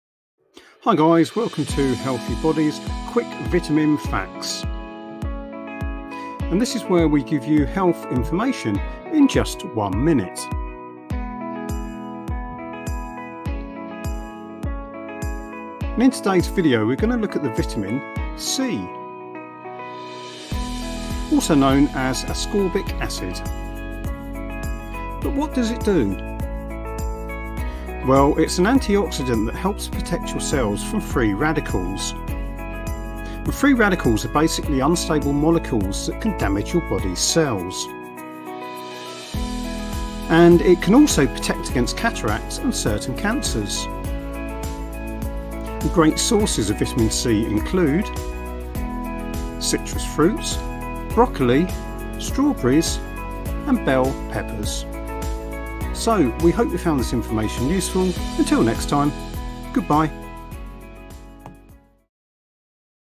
Vitamin C Facts
In this video we look at Vitamin C facts, we explain what Vitamin C does and also how you can get Vitamin C naturally in certain foods.